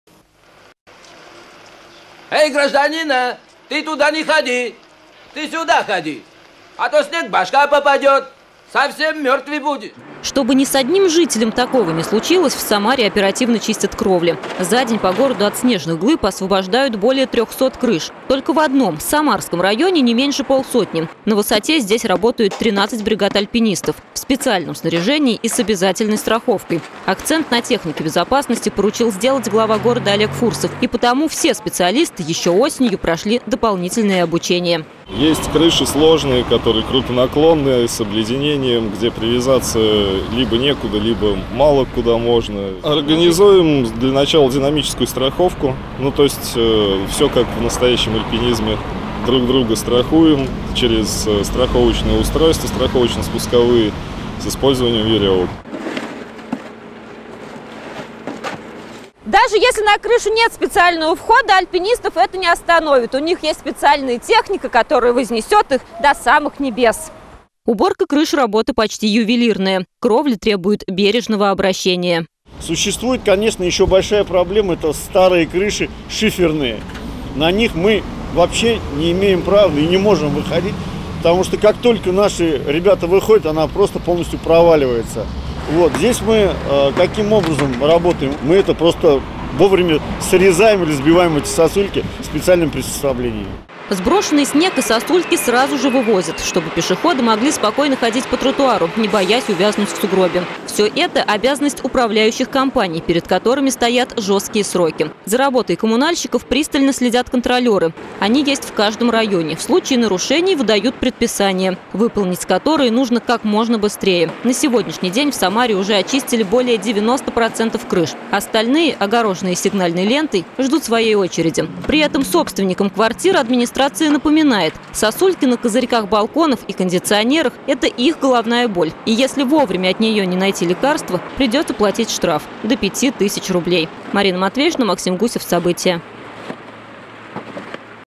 Уборка придомовой территории и скатных кровель
В Самаре очищают крыши от снега и наледи - рабочим нужно уложиться в самые короткие сроки. На сегодня очищены уже 90 процентов крыш